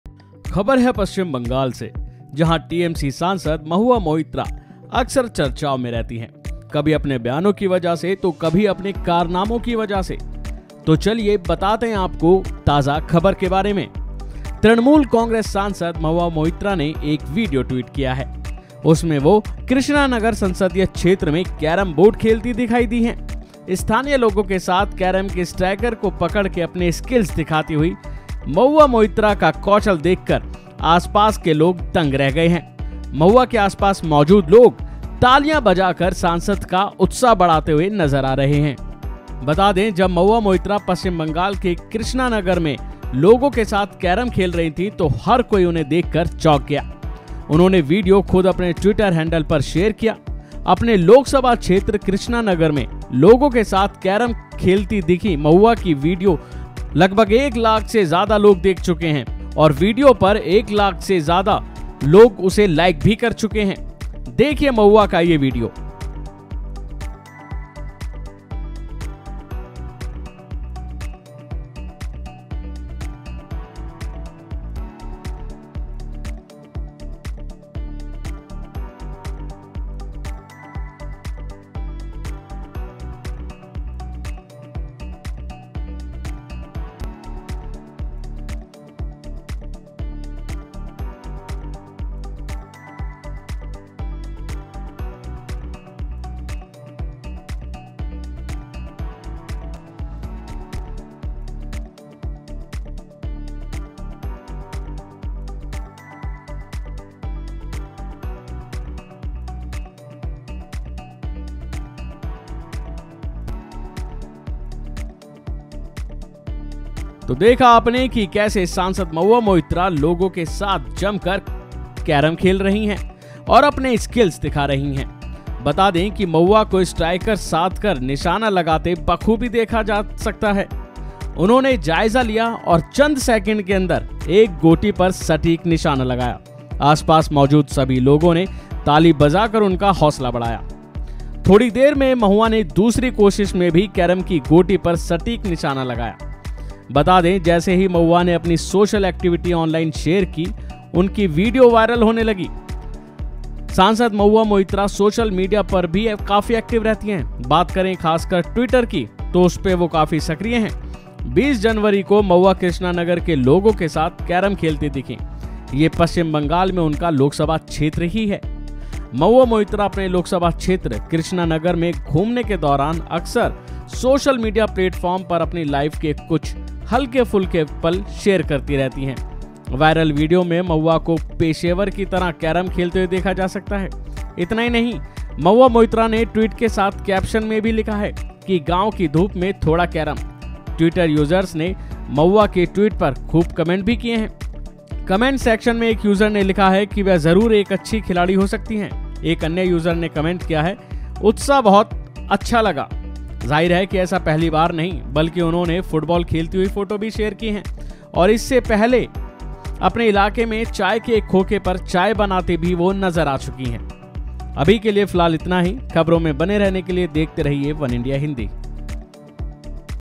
West Bengal: TMC सांसद Mahua Moitra दिखीं Carrom खेलती हुई, Video Viral | वनइंडिया हिंदी
खबर है पश्चिम बंगाल (West Bengal) से जहां टीएमसी सांसद (TMC MP) महुआ मोइत्रा (Mahua Moitra) का वीडियो वायरल हो रहा है। महुआ मोइत्रा ने जो वीडियो ट्वीट की है उसमें वे कृष्णानगर (Krishna Nagar) संसदीय क्षेत्र में कैरमबोर्ड (Mahua Moitra Carrom Board Video)  खेलते दिखीं।